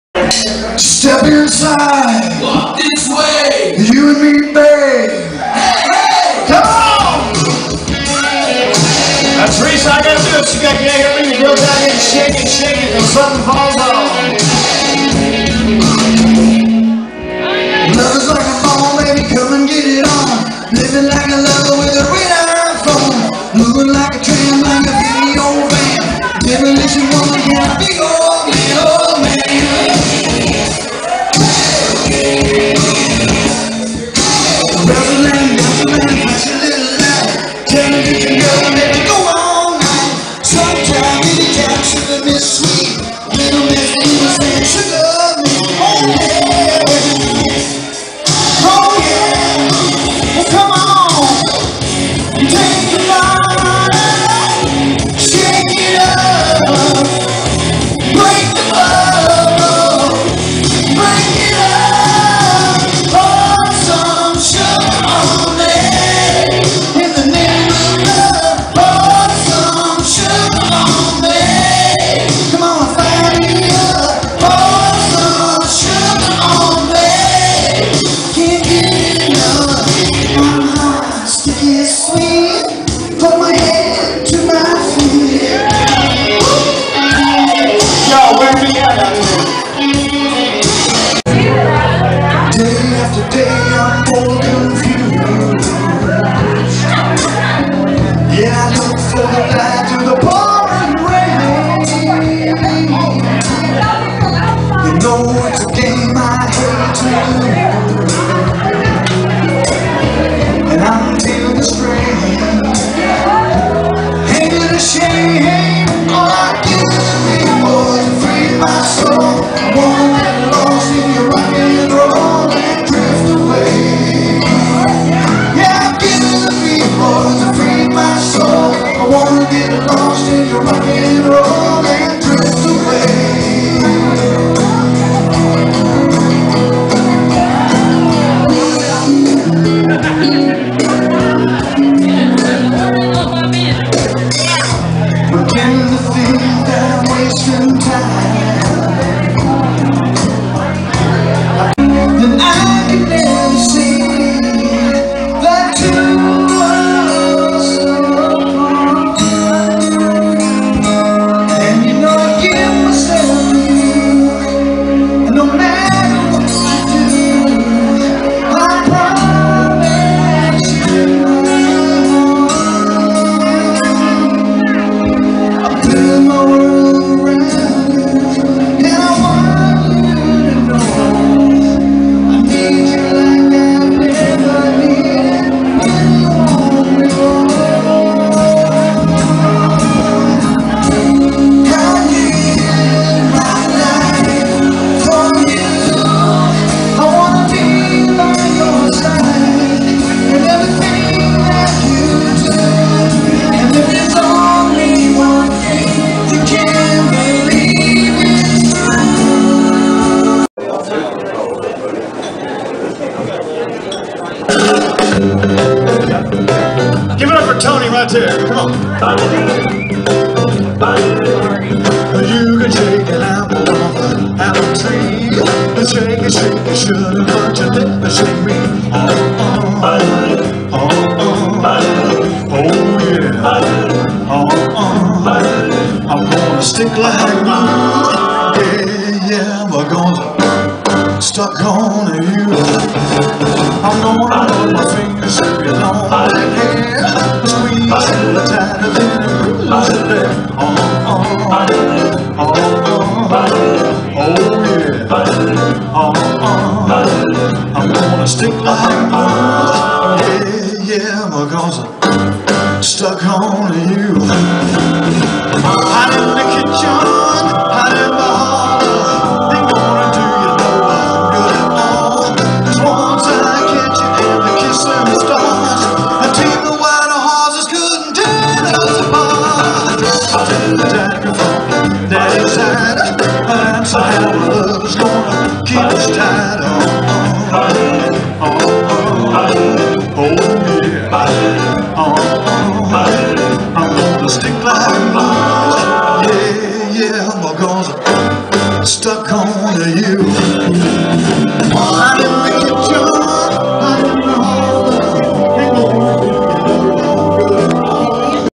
Concert Melody Live
Here is a short melody of a few songs done live several years ago from a few different live performances